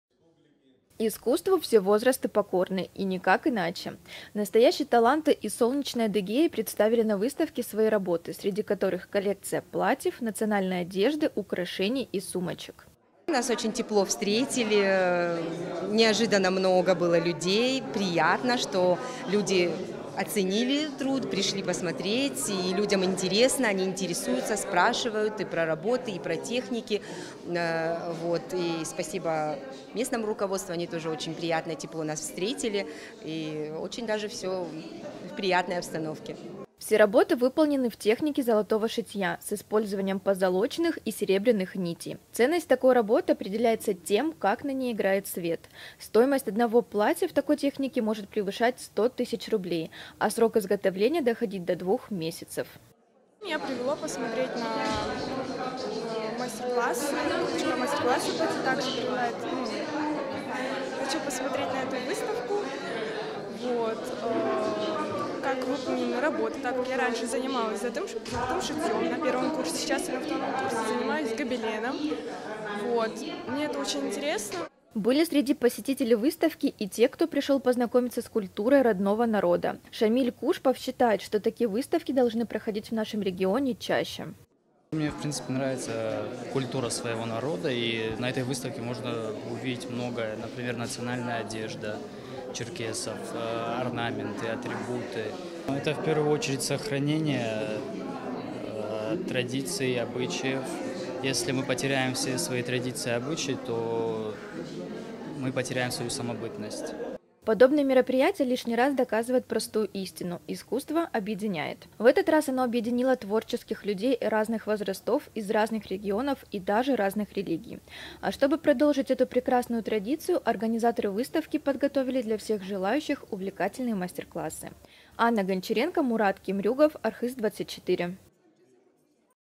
«Промыслы и ремесла Адыгеи» приехали в Черкесск
В рамках совместного празднования 100-летия Карачаево-Черкесии и Республики Адыгея, наши соседи по СКФО подготовили для жителей КЧР особую программу. Например, в Картинной галерее Черкесска они развернули выставку «Промыслов и ремесел Адыгеи», представив на ней лучшие работы народных мастеров. Сюжет о которой выходит в нашей традиционной праздничной рубрике «Про100».